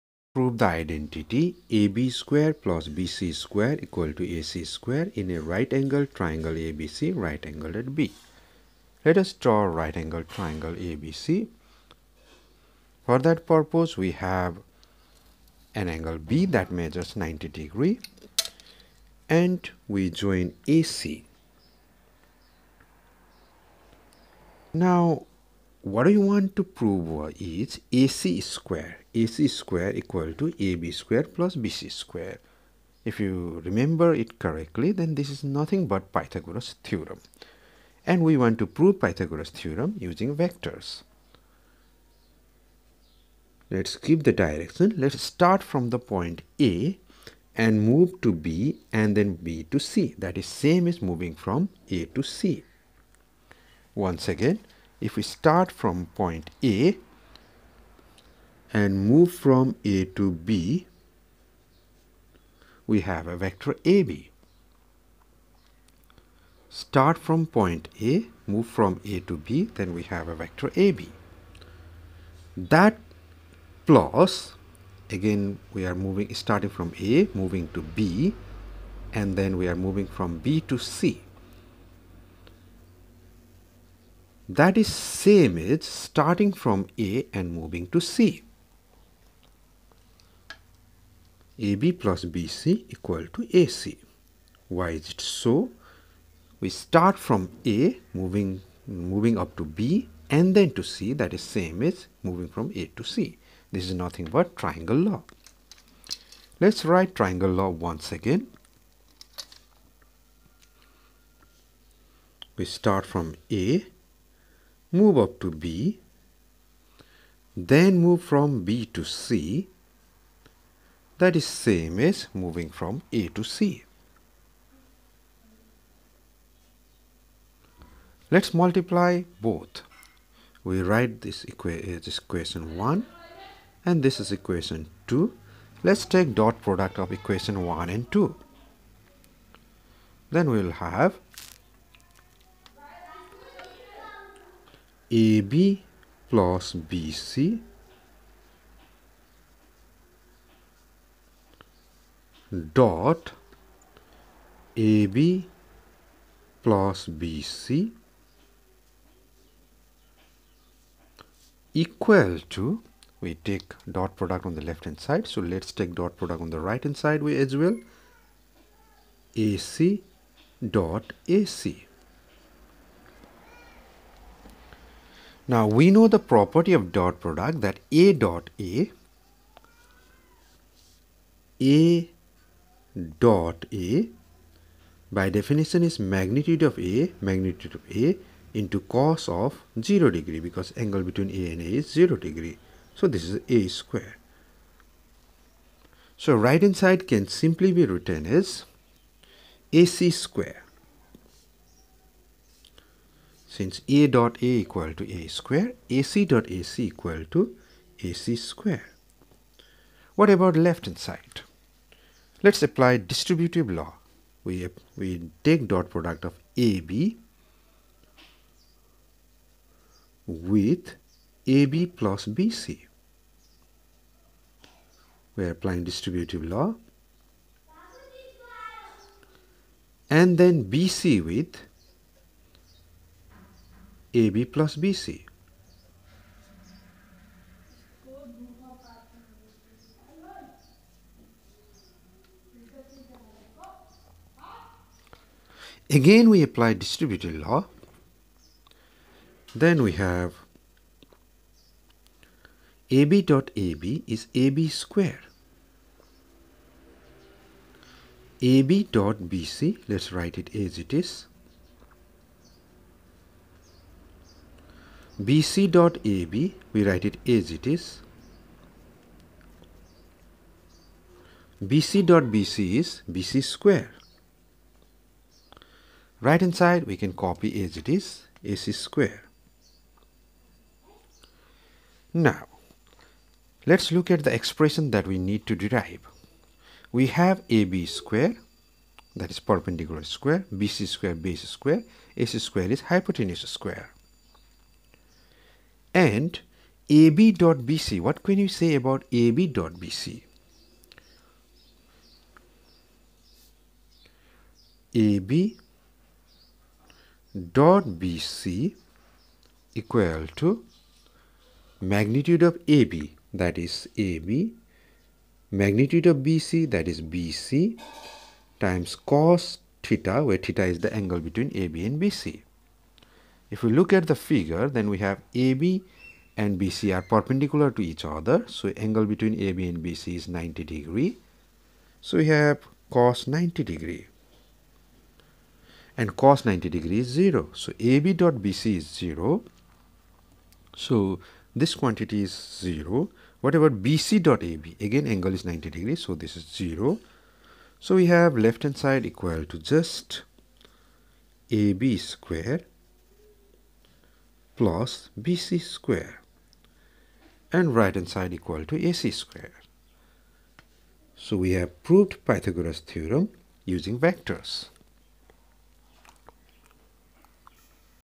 Proof of Pythagorean theorem using vector method
We prove Pythagoras theorem using dot product of vectors.